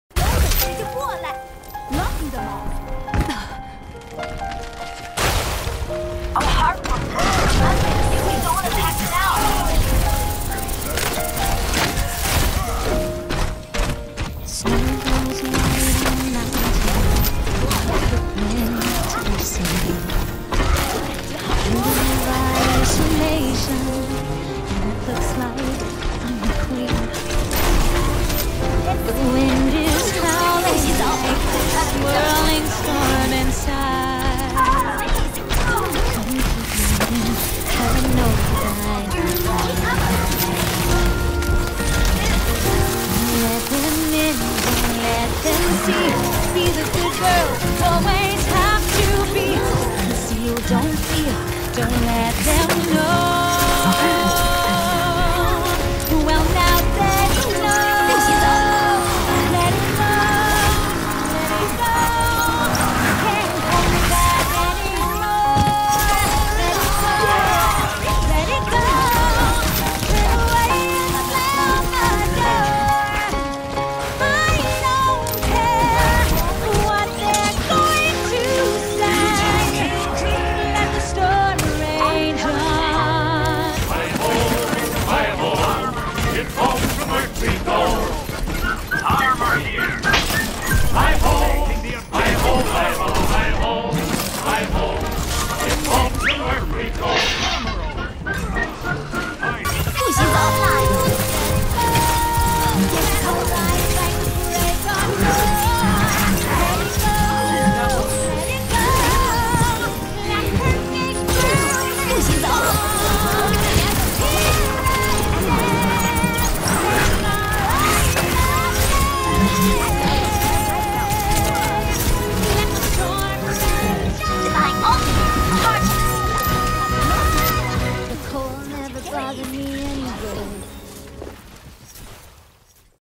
An A-Mei-zing Troll Stream
This is what happens when Cuzi, Wes, and I feel like cracking each other up for an evening.